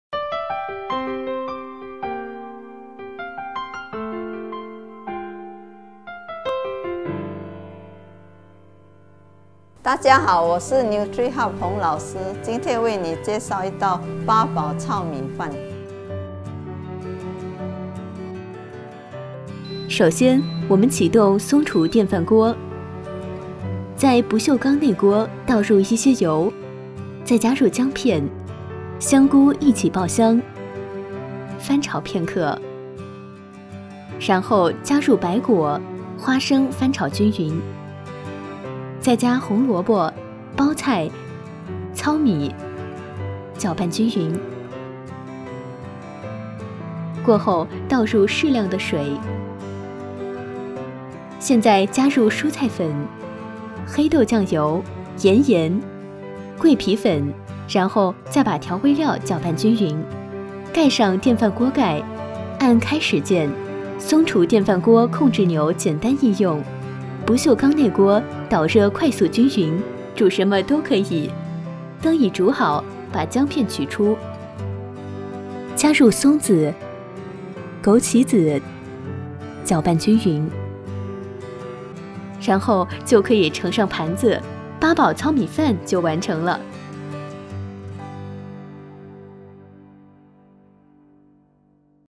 Song-Cho NUTRI-HEALTHY COOKING - 8 Treasure Brown Rice (Song-Cho Rice Cooker)
Cook a healthy and satisfying "one-pot" 8 Treasure Brown Rice using Song-Cho Rice Cooker (SC-FCM58) with stainless steel inner pot. Full Recipe (ENG/CHI)